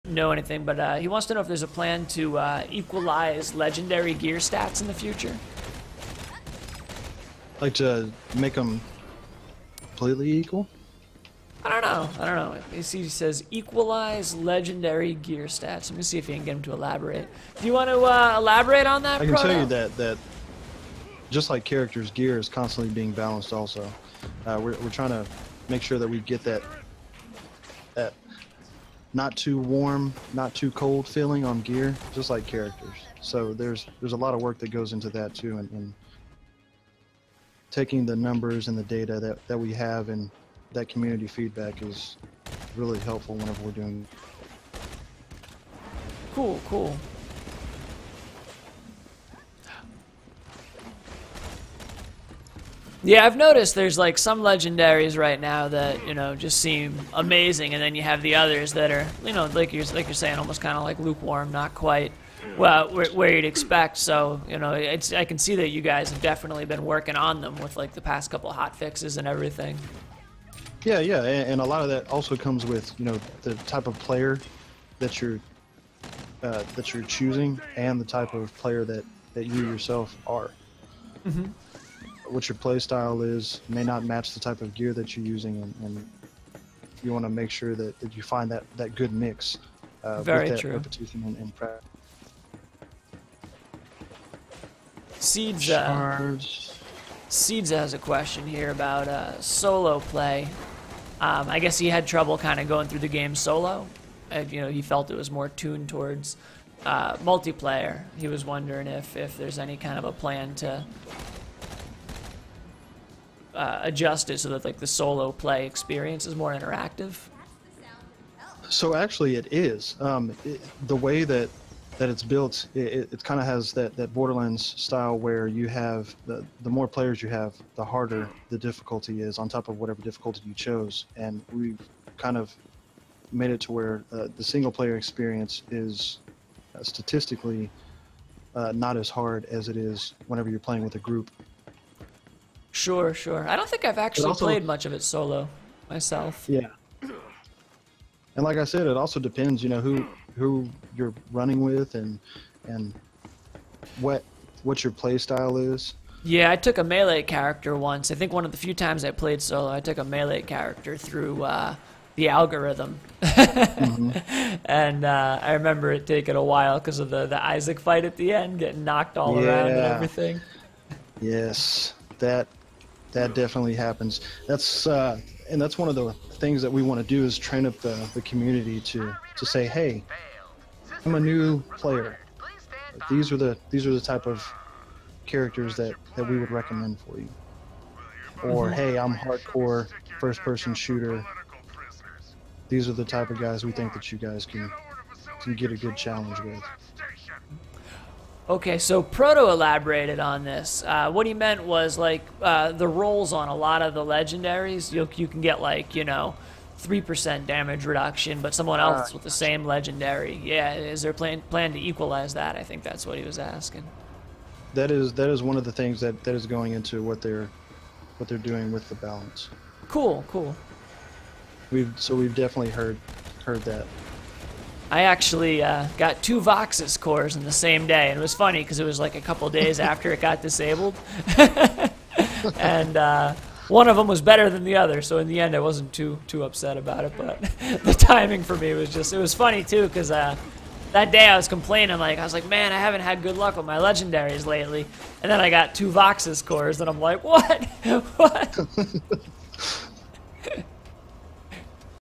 Anthony Nicholson talks on Gear & game balance.
The Battleborn Associate Producer Anthony Nicholson answers a few questions about game and gear balance.